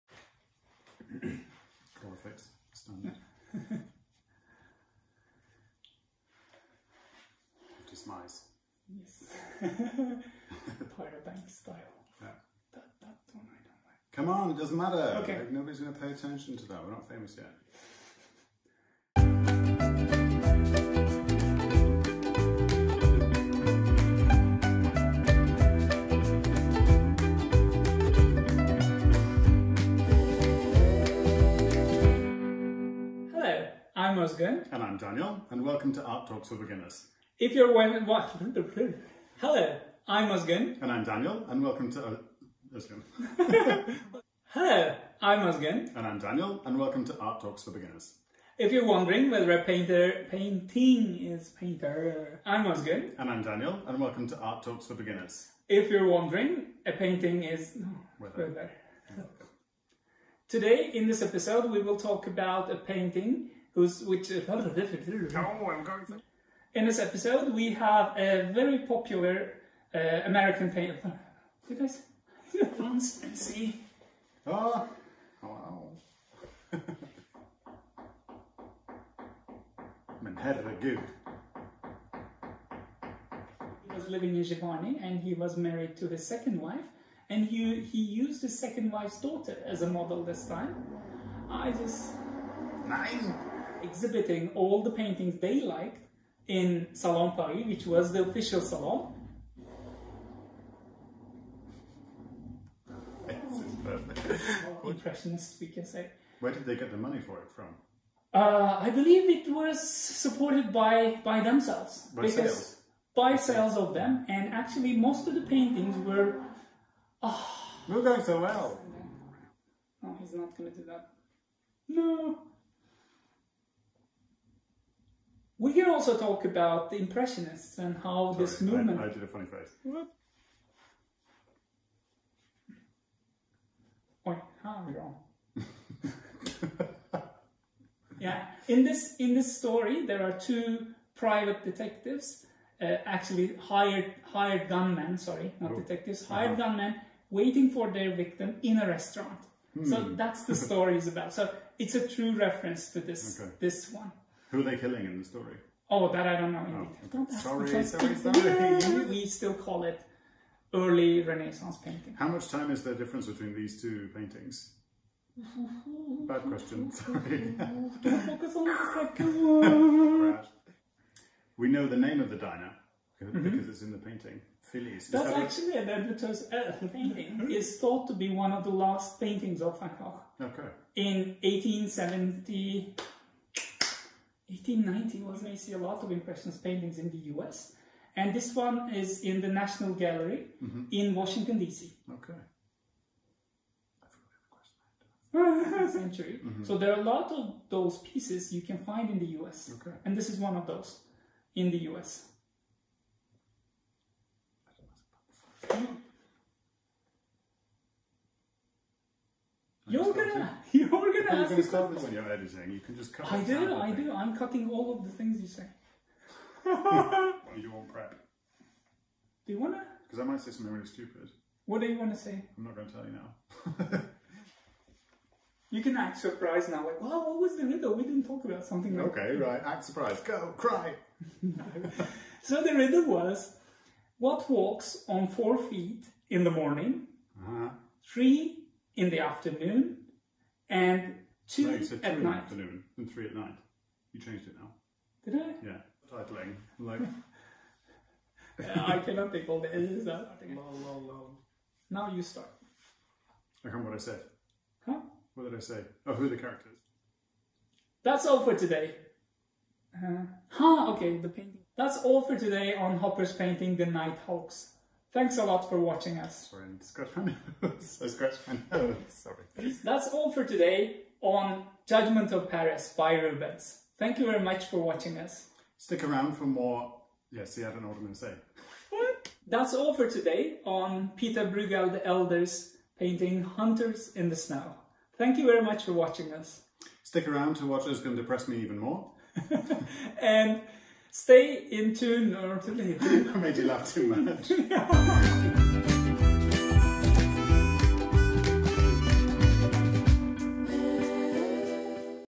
Bloopers No.2 | Art Talks for Beginners!
Bloopers from
Art Talk for Beginners! Episodes:
No.5 Millais - Ophelia
No.7 Botticelli - The Allegory of Spring
No.8 Monet - Woman with a Parasol
No.9 Pieter Brueghel the Elder - Hunters in the Snow
No.10 Van Gogh - Wheatfield with Crows
No.11 Moreau - Oedipus and the Sphinx
No.12 Rubens - Judgement of Paris
No.14 Hopper - The Nighthawks